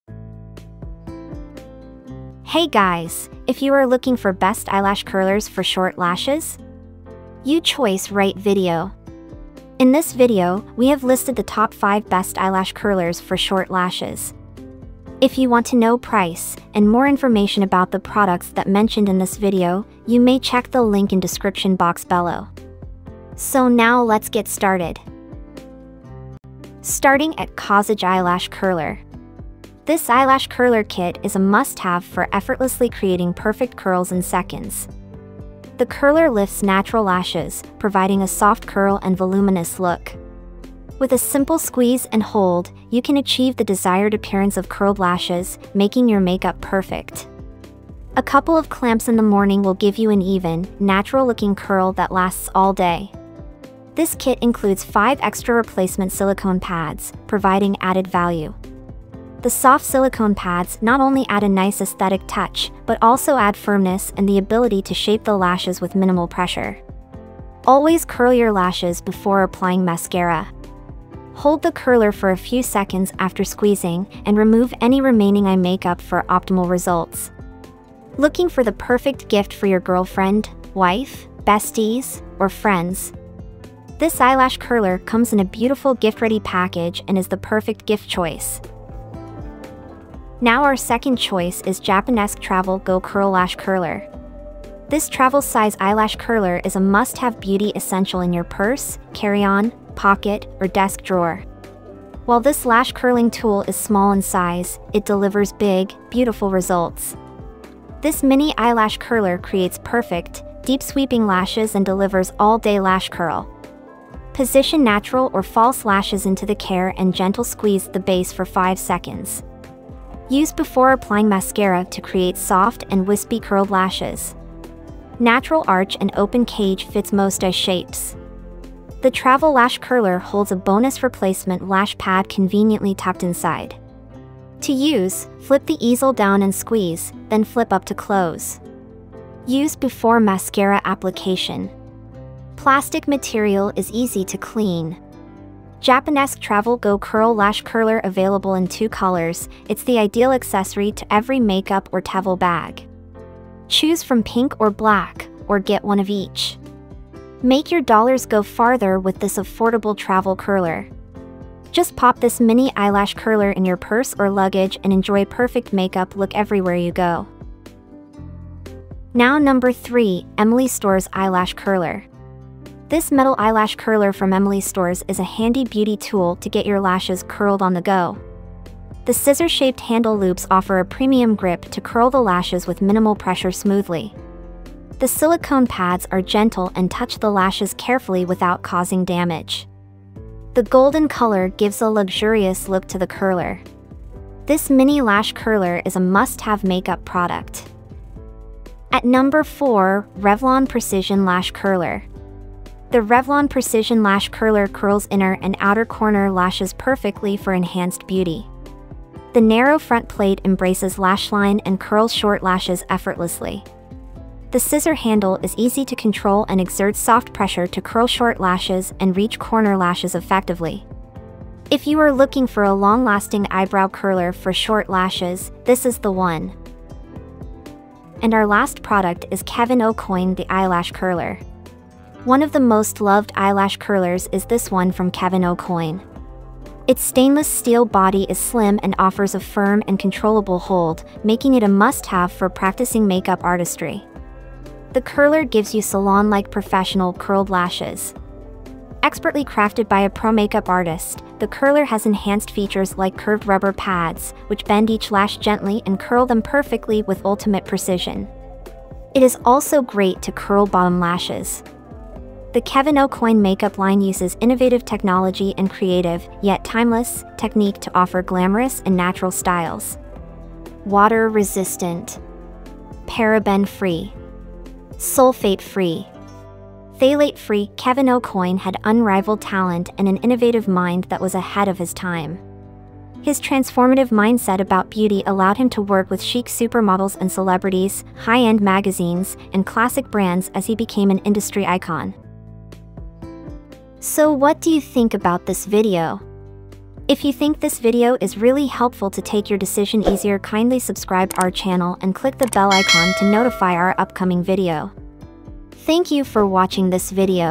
5 Best Eyelash Curlers For Short Lashes
Links to the Best Eyelash Curlers For Short Lashes 2024 we listed in this video:
1.Kaasage Eyelash Curler
2.JAPONESQUE Travel Go Curl Lash Curler
3.EMILYSTORES Eyelash Curler
4.Revlon Precision Lash Curler
5.Kevyn Aucoin The Eyelash Curler

In this video, we’ll be comparing the Best Eyelash Curlers For Short Lashes that are designed for different kinds of users. We will take into consideration performance, features, and price; so you can decide which is best for you.  All the products on our list were selected based on their own inherent strengths and features.
We’ll break down which Best Eyelash Curlers For Short Lashes is best for you, and what you can expect to get in return for your money.  We’ll help you decide if one of the models on our list seems like a great purchase.

Music

SONG

ARTIST

ALBUM